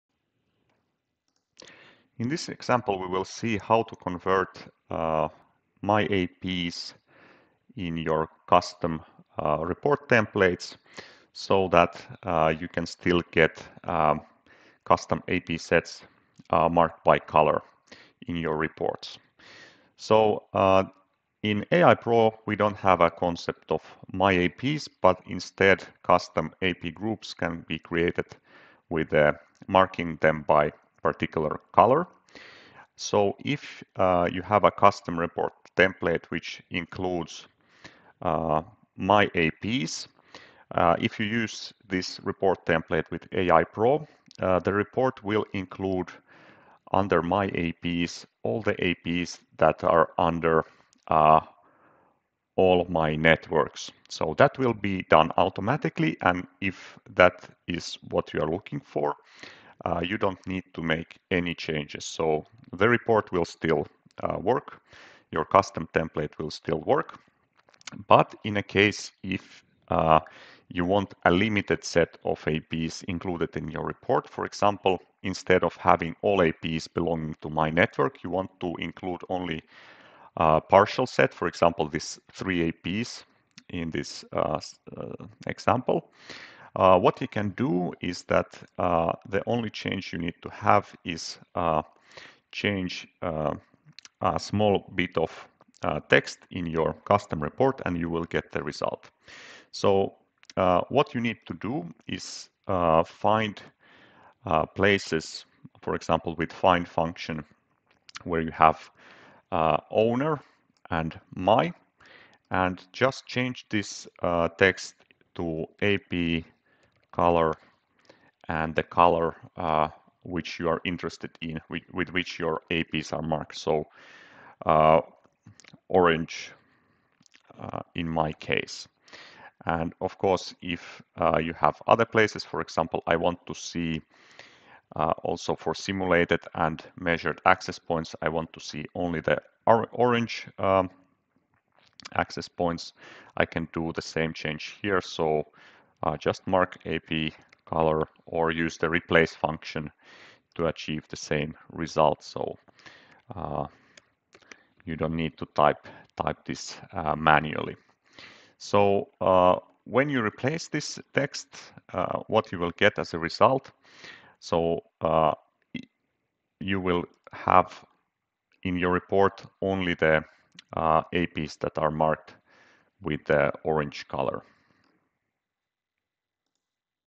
Ekahau AI Pro | Custom Report Templates Overview | My APs and Color Marked APs
AI Pro introduces the “My Networks” concept for more effective use with most common Wi-Fi planning cases and thus the concept of My APs that was available in its full form till Pro 10.4 is replaced with the new functionality.

If you have some custom report templates in use that rely heavily on custom “My APs” groups that do not respond directly to the set of APs belonging to the same network, you can still reuse these report templates with a slight modification to your custom AP groups.

Case 1: I had a project in Pro 10 that involves “My APs” and all of these APs are related to My Networks (i.e. all APs of your networks/SSIDs are marked as “My APs”). I want to use my custom report template which contains My APs.

Solution: You can use your current custom template as such without any changes, because when you generate a report with this template from AI Pro, this report will include all APs belonging to My Networks in the place of My APs.

Case 2: I had a project in Pro 10 that involves custom AP groups marked as “My APs” and these do not directly correspond to APs related to My Networks.

Solution: You can reuse your current custom report templates with a slight change to the AP group markings. What you need is:
1. Mark APs belonging to your custom AP group (custom group of “My APs”) with a particular color (e.g. orange). This can be done by selecting all APs you want to mark and assigning color to them
2. Open your custom report template with “My APs” and use find and replace function to replace the sections where you want to have the custom AP group marked with color.
3. Save the template and now your report will include custom AP groups marked with orange color (or whatever color you decide to use).